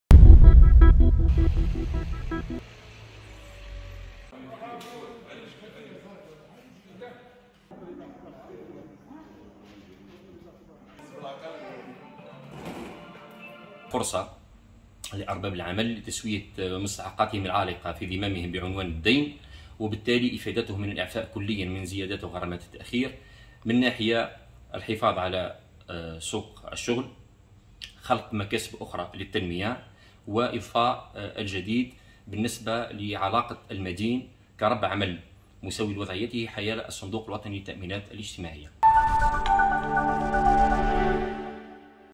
تبسة: إعفاء قرابة 5.200 مستخدم مدين منتسب للضمان الاجتماعي من عقوبات التأخير‎
تبسة (وأج)- أحصى الصندوق الوطني للتأمينات الاجتماعية (وكالة تبسة) قرابة 5.200 مستخدم منتسب لدى الضمان الاجتماعي معني بتدابير الإعفاء من العقوبات والغرامات المالية الناجمة عن التأخر في تسديد الاشتراكات.